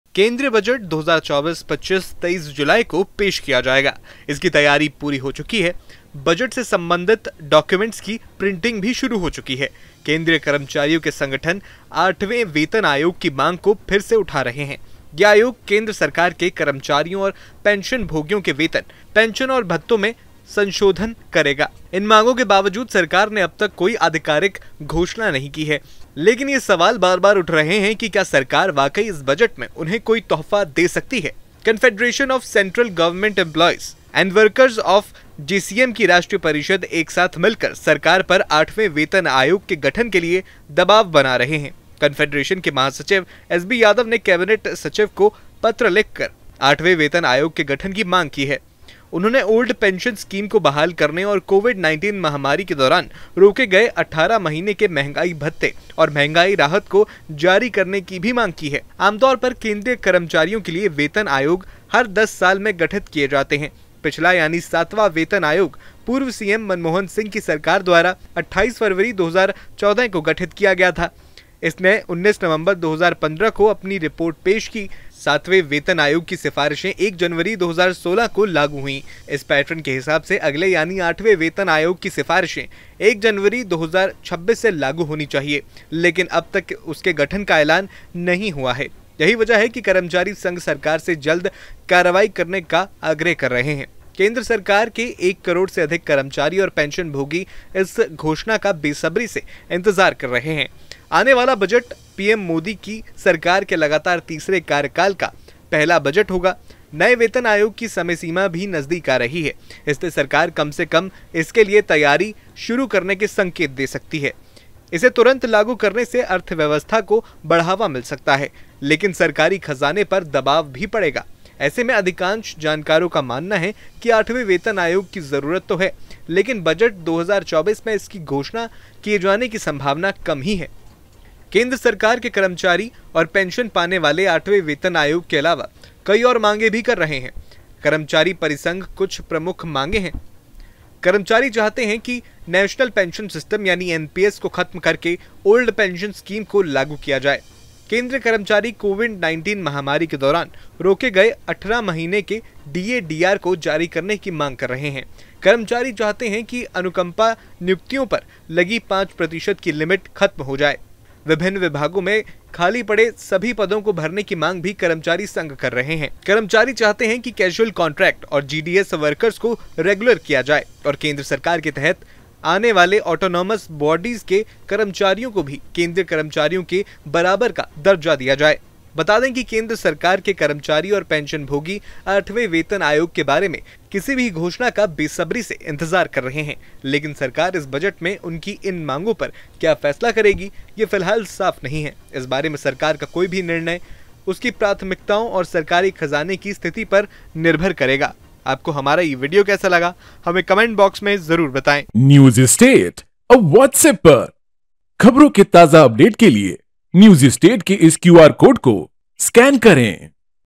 08th Pay Commission: केन्द्रीय कर्मचारियों के लिए बड़ी खुशखबरी, Union Budget 2024 में ऐलान संभव
08th Pay Commission: केन्द्रीय कर्मचारियों के लिए बड़ी खुशखबरी, Union Budget 2024 में ऐलान संभव

केंद्रीय बजट 23 जुलाई को 2024-25 पेश किया जाएगा। इसकी तैयारी पूरी हो चुकी है। बजट से संबंधित डॉक्यूमेंट्स की प्रिंटिग भी शुरु हो चुकी है।केंद्रीय कर्मचारियों के संगठन 8वें वेतन आयोग की मांग को फिर से उठा रहे हैं। ये आयोग केंद्र सरकार के कर्मचारियों और पेंशनभोगियों के वेतन, पेंशन और भत्तों में संशोधन करेगा। इन मांगों के बावजूद, सरकार ने अब तक कोई आधिकारिक घोषणा नहीं की है। लेकिन ये सवाल बार-बार उठ रहे हैं कि क्या सरकार वाकई इस बजट में उन्हें कोई तोहफा दे सकती है?

News State Apps डाउनलोड करें:
Download Android and iOS Apps for more updates: